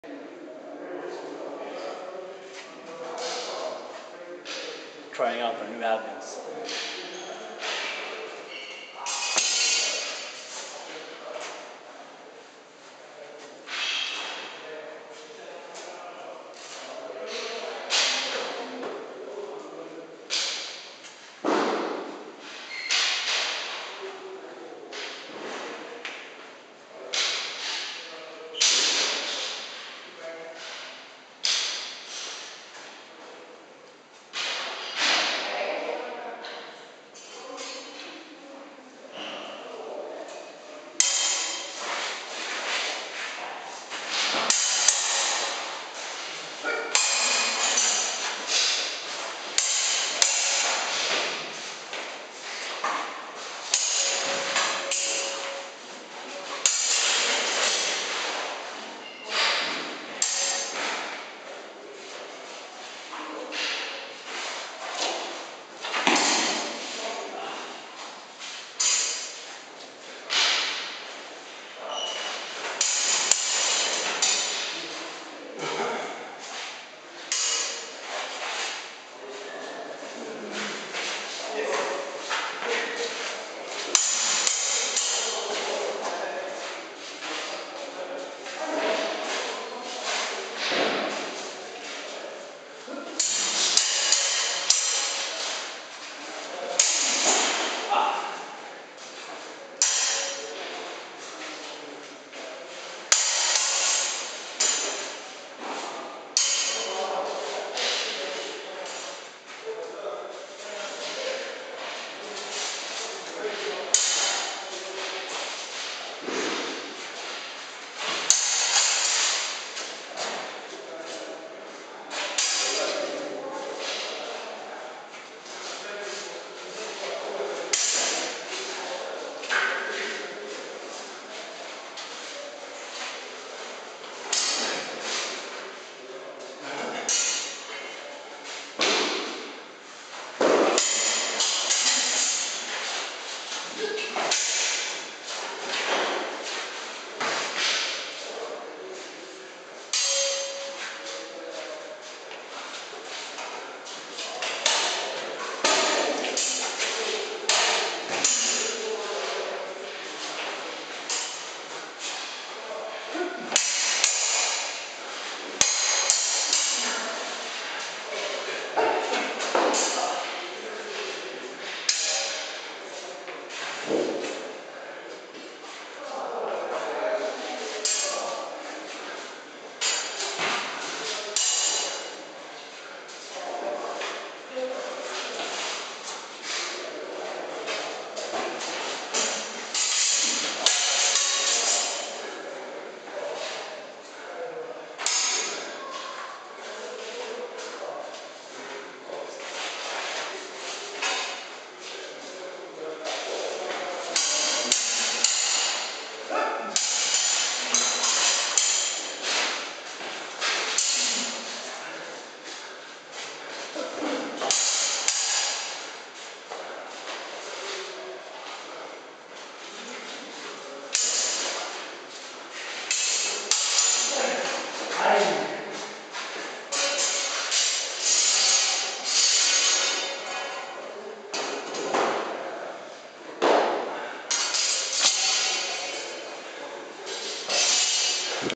Axel vs Scott steel sparring European martial arts
Axel (red shirt) and Scott (black/white) spar with Albion Meyers at GHFS